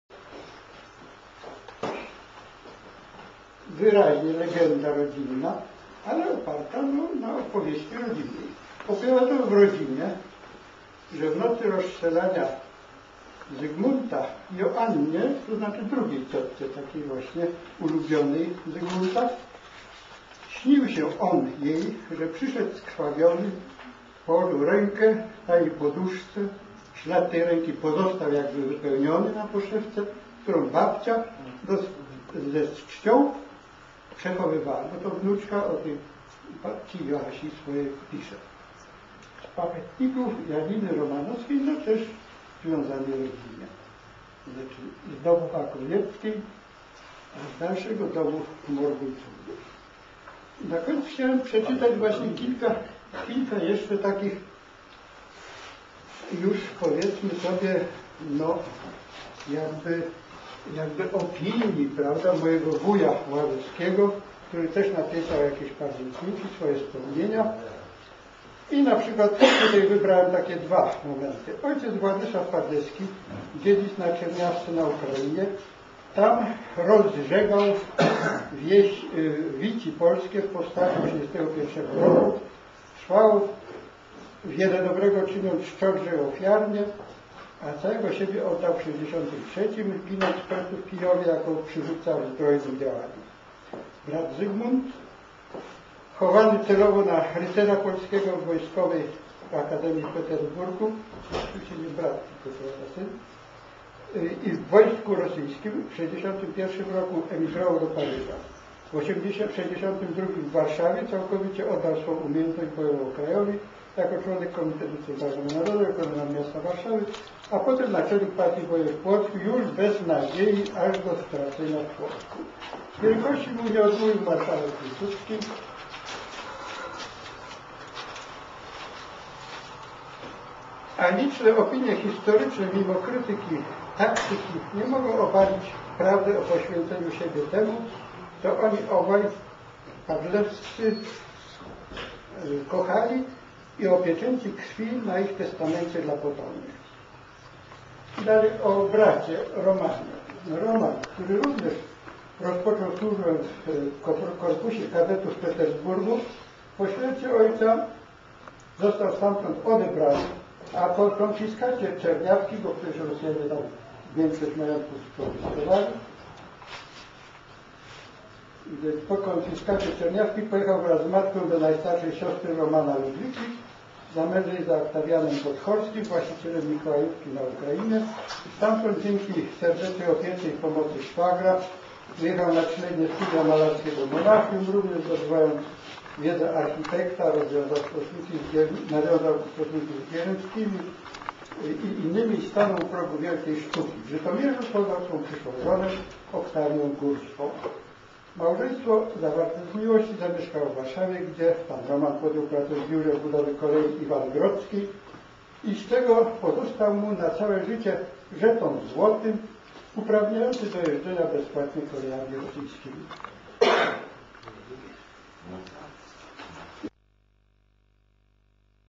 Sen ciotki Padlewskiego J Morgulec 97r Autor Zenon Dylewski
Członek  Rodziny, opowiada  młodzieży  z  Małachowanki  o  losach   gen.Z.Padlewskiego .  Spotkanie  w  Spichlerzu.